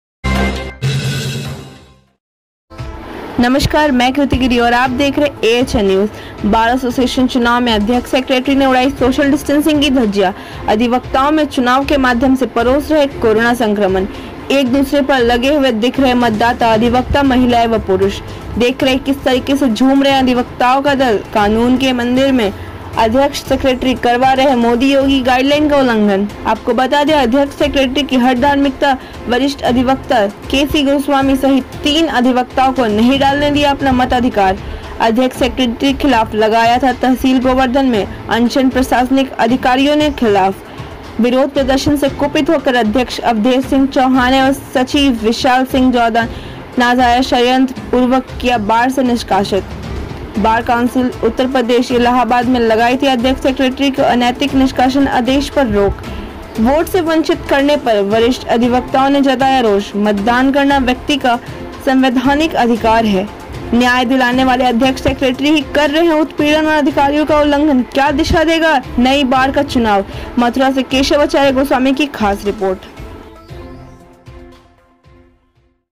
बार एसोसिएशन चुनाव में अध्यक्ष सेक्रेटरी ने उड़ाई सोशल डिस्टेंस की धज्जियां
अधिवक्ताओं में चुनाव के माध्यम से परोस रहे हैं कोरोना संक्रमण

एक दूसरे पर लगे हुए दिख रहे हैं मतदाता अधिवक्ता महिला एवं पुरुष

देख रही है किस तरह झूम रहा है अधिवक्ताओं का  दल

कानून के मंदिर में अध्यक्ष सेक्रेटरी करवा रहे हैं मोदी योगी के गाइडलाइन का उल्लंघन

 अध्यक्ष सेक्रेटरी की हठधर्मिता वरिष्ठ अधिवक्ता के सी गोस्वामी सहित तीन अधिवक्ता को नहीं डालने दिया अपना मताधिकार

 अध्यक्ष सेक्रेटरी के खिलाफ लगाया था तहसील गोवर्धन में अनशन प्रशासनिक अधिकारियों के खिलाफ

  विरोध प्रदर्शन से कुपित होकर अध्यक्ष  अवधेश सिंह चौहान एवं सचिव विशाल सिंह जादौन नाजायज षडयंत्र पूर्वक किया   बार से निष्कासन

 बार काउंसिल उत्तर प्रदेश इलाहाबाद में लगाई थी अध्यक्ष सेक्रेटरी के अनैतिक निष्काशन आदेश पर रोक

वोट से वंचित करने पर वरिष्ठ अधिवक्ताओं ने जताया रोष

मतदान करना व्यक्ति का संवैधानिक अधिकार

न्याय दिलाने वाले अध्यक्ष सेक्रेटरी ही कर रहे हैं उत्पीड़न और अधिकारों का उल्लंघन क्या दिशा देगा नई बार का चुनाव